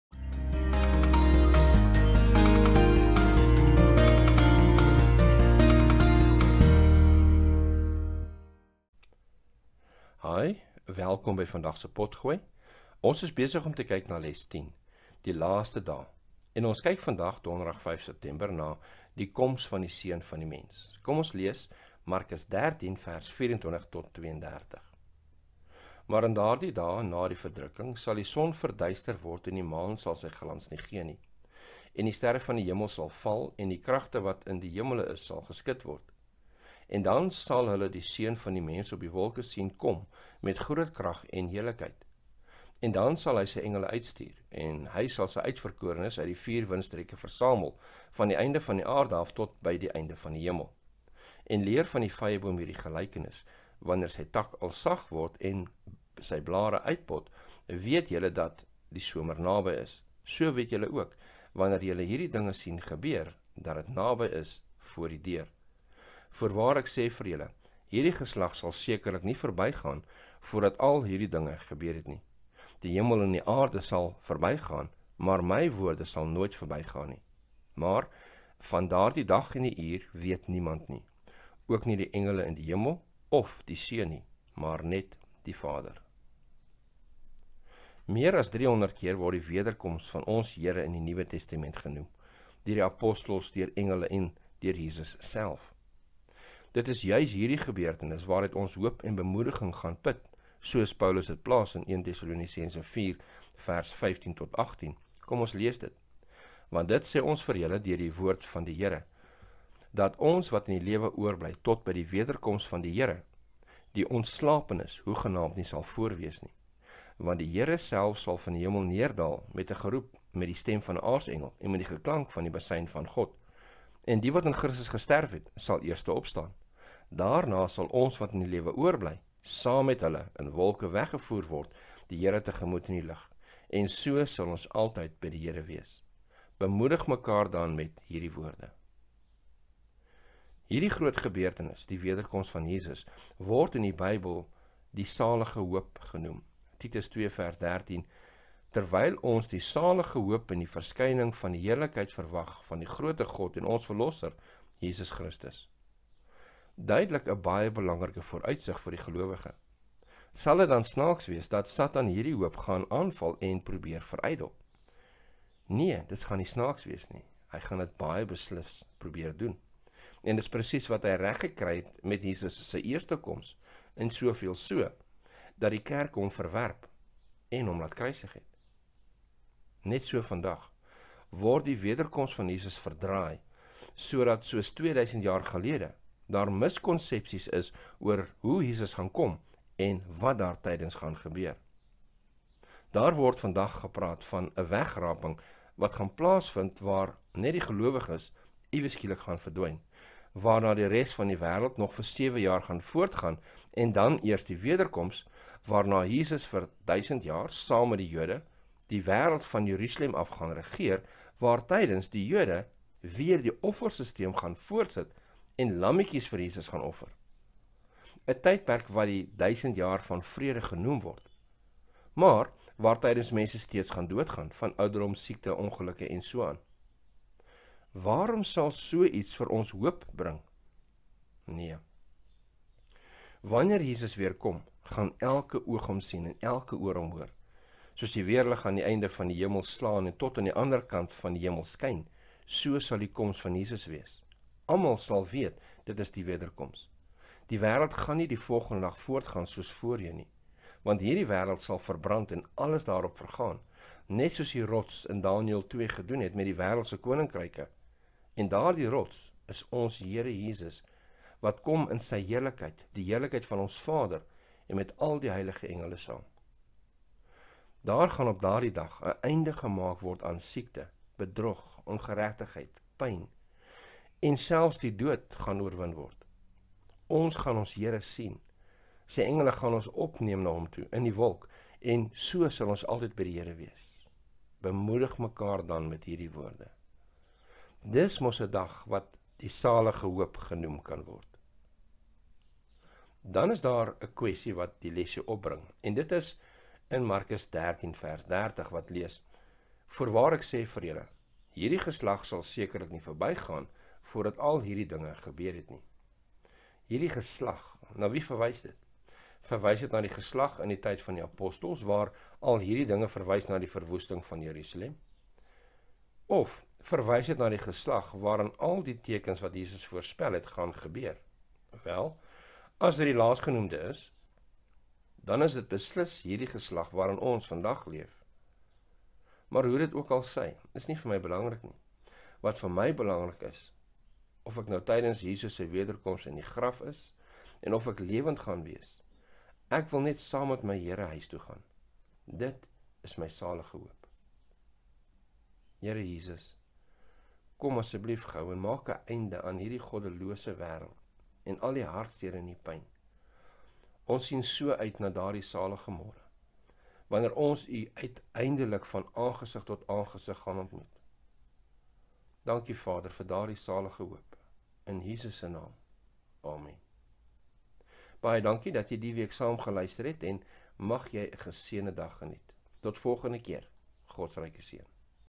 Donderdag 05 Sept. - ‘Die koms van die Seun van die Mens’
Les 10 –  Die laaste dae
31 Augustus tot 06 September.

Derde Kwartaal 2024 – DIE EVANGELIE VOLGENS MARKUS

Aanbieder:
David Kühn

CREDITS:
CREDITS: Alle regte is die van die Wereld Konferensie van die Sewende-dag Adventiste Kerk.
Jesus Christus.